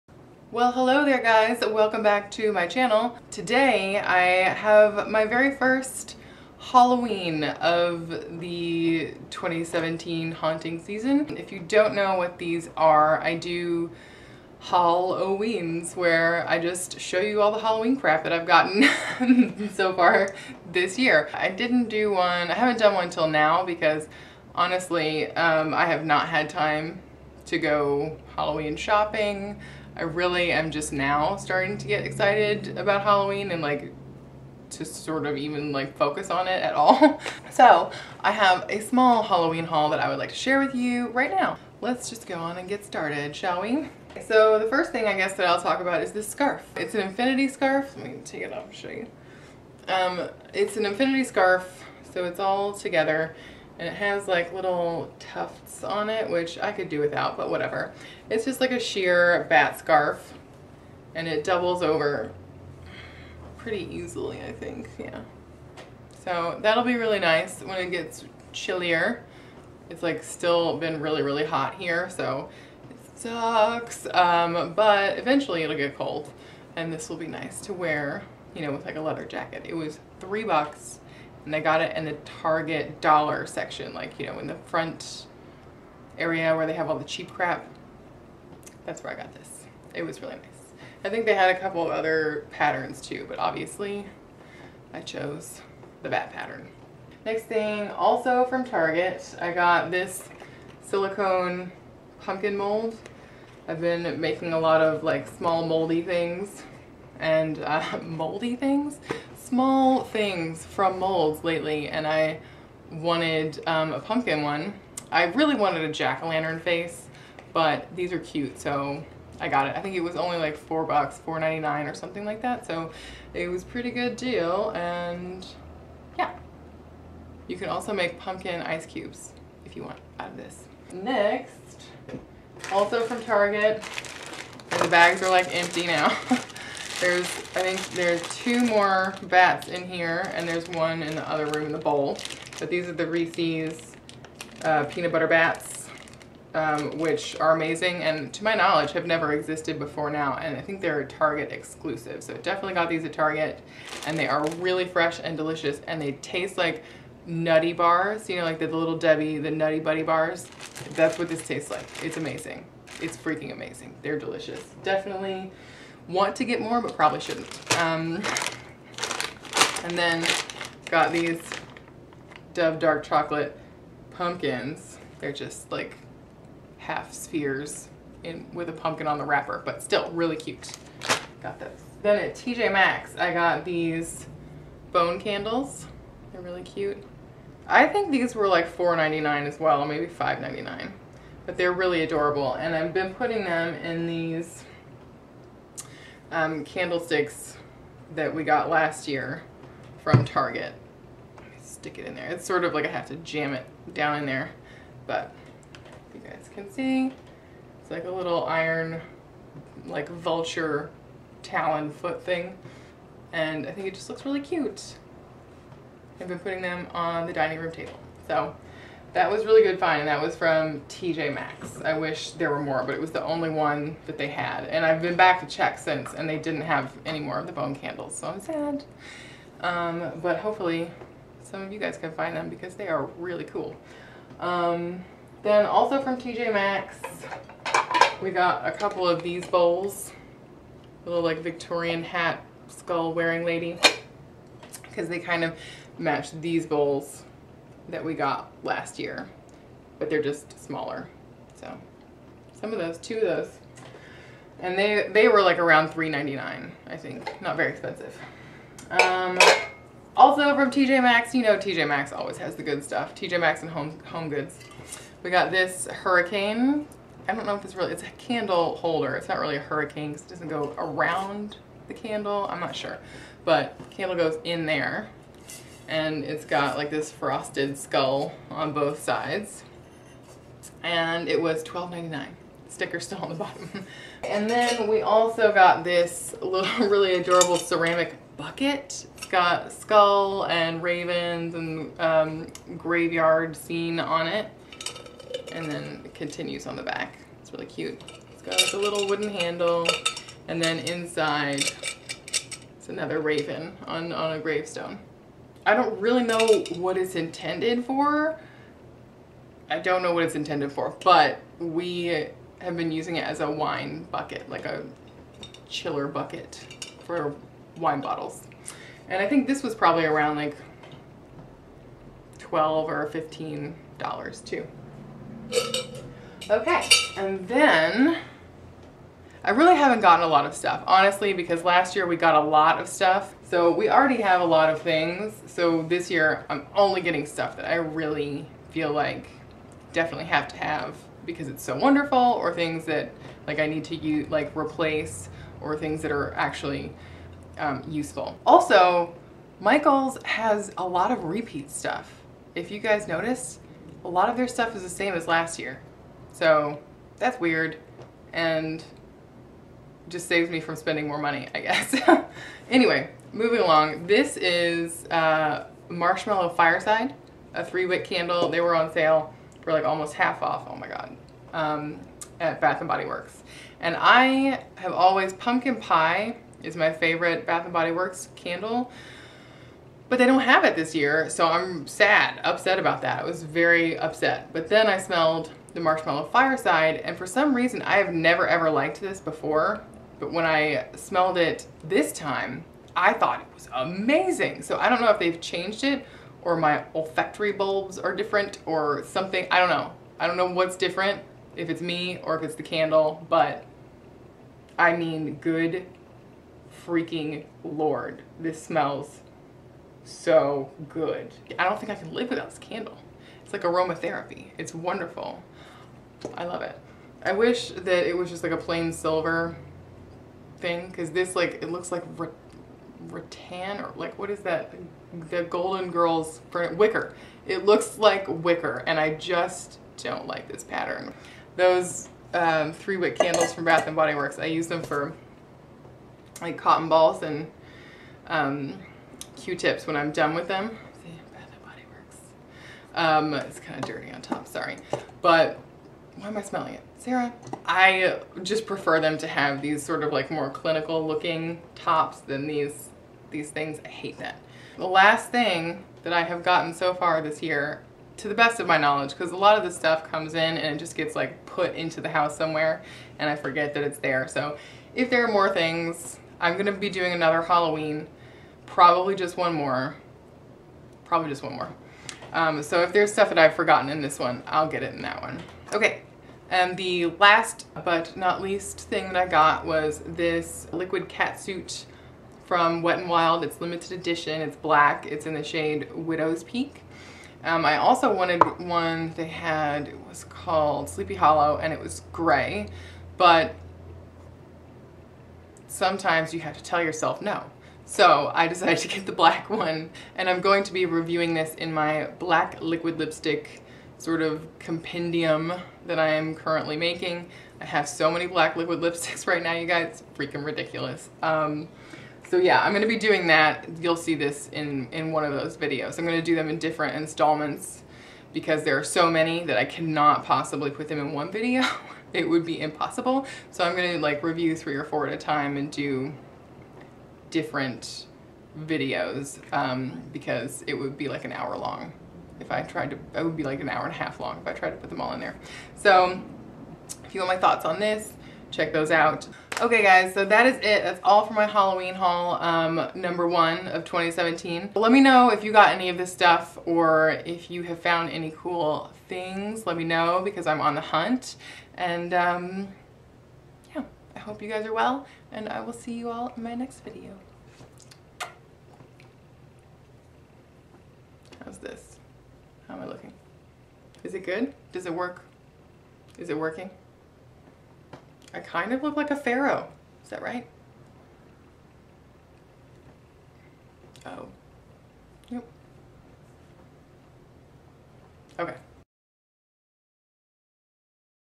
HAUL O WEEN | SEPTEMBER! Spooky Decor from Target, TJ Maxx, Homegoods
Hey Fiends!
  It's that time of year again, September, when all the normal places start to carry their spooky Halloween decor, so you know I've been doing a bit of shopping. Here's my little decor and home wares haul so far from Target, TJ Maxx and Homegoods. I haven't gotten anything from Michael's or Jo Ann craft stores, though I do plan on doing at least one more of these, so may see some more haunting decoration from those place in a future video!

xx,
the Saramonster